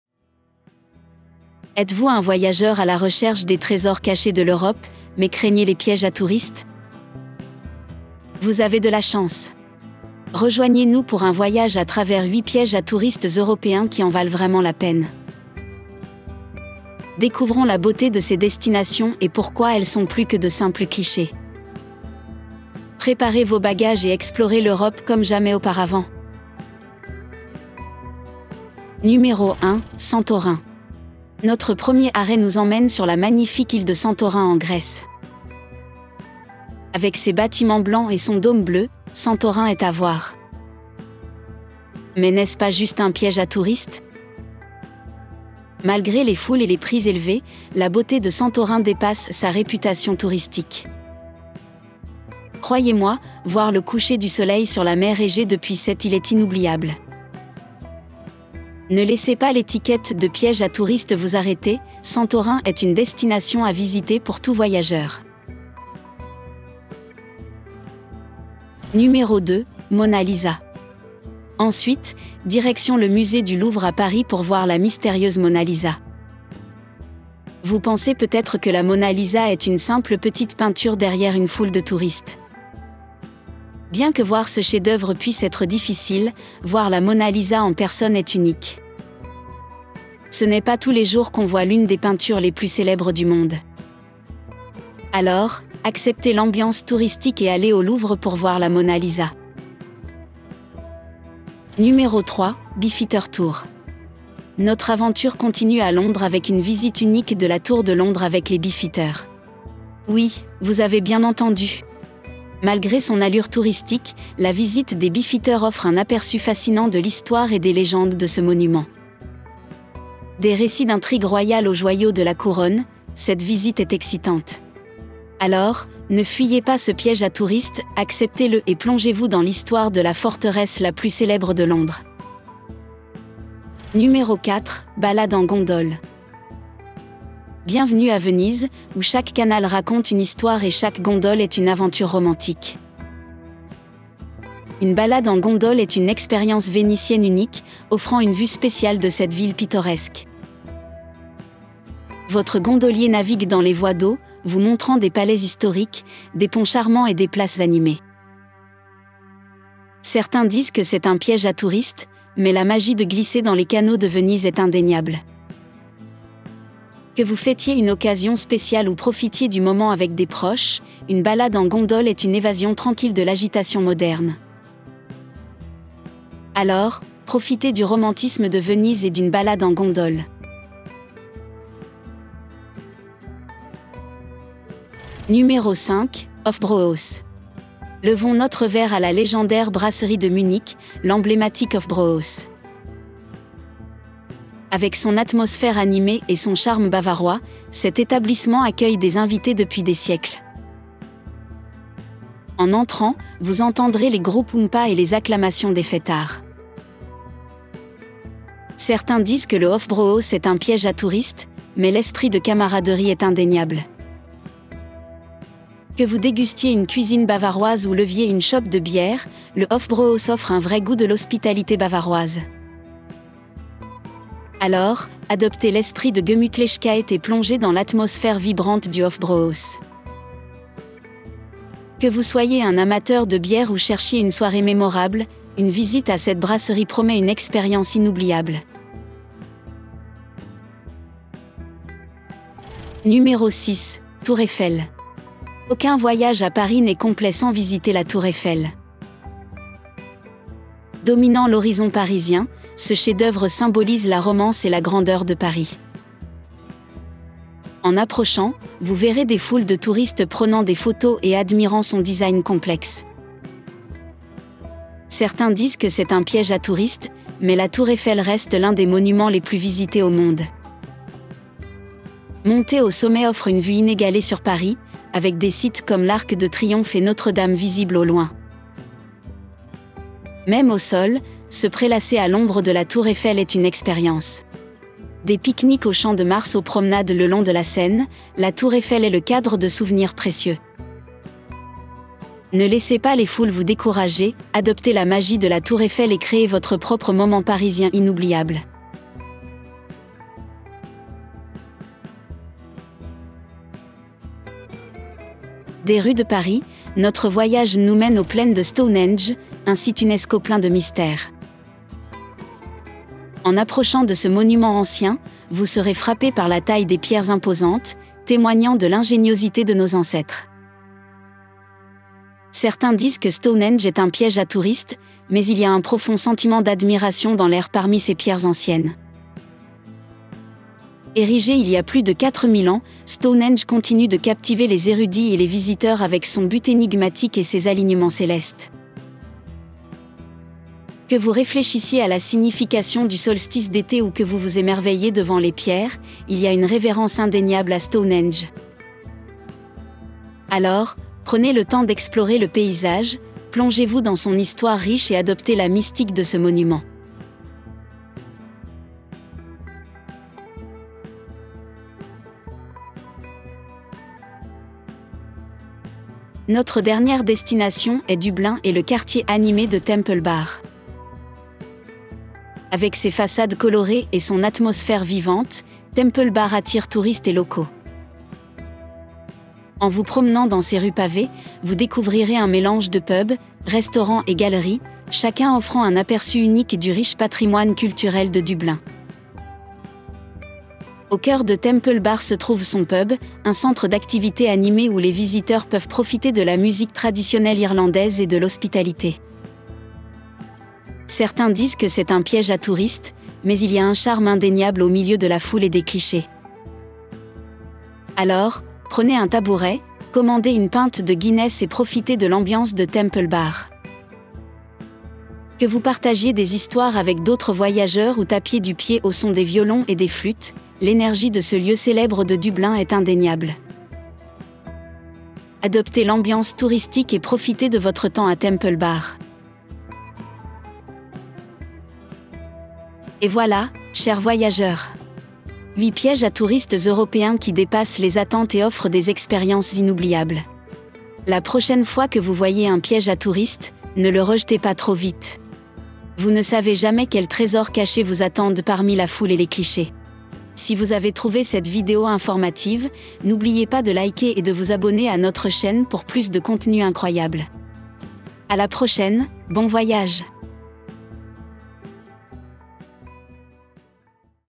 8 Pièges à Touristes qui en Valent la Peine en Europe (2024)
Découvrez les trésors cachés d'Europe avec nous ! Dans cette vidéo, nous vous emmenons à travers huit destinations européennes souvent considérées comme des pièges à touristes, mais qui valent vraiment le détour. De la magnifique île de Santorin à la mystérieuse Stonehenge, en passant par la légendaire Tour Eiffel et l'animé quartier de Temple Bar à Dublin, chaque arrêt vous surprendra par sa beauté et son charme unique.

Vous pensez que ces endroits sont surcotés ? Laissez-nous vous prouver le contraire ! Préparez-vous à explorer l'Europe comme jamais auparavant et à découvrir pourquoi ces destinations sont bien plus que de simples clichés touristiques.